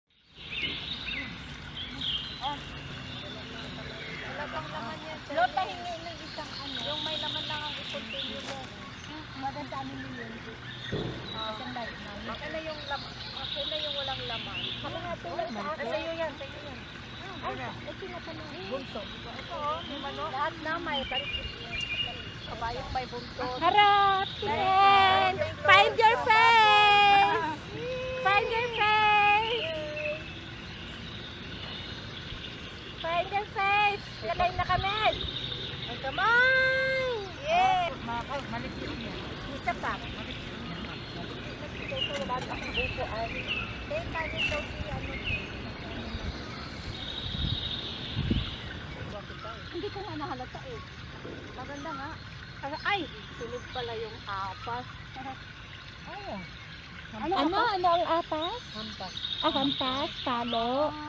20F BONDING ZÜRICH SWITZERLAND MAY2019
20F bonding
mga katagang sariling katha, "selfie, find ur face", "ay kabayo!", "ang kamayyyyyyyy", mga katagang ng iba ibang lugar na may kakaibang kahulugan. simpleng kasiyahan sa buhay na mananatili sa ating kaisipan dumating man ang panahon na hindi na natin ito kaya pang gawin...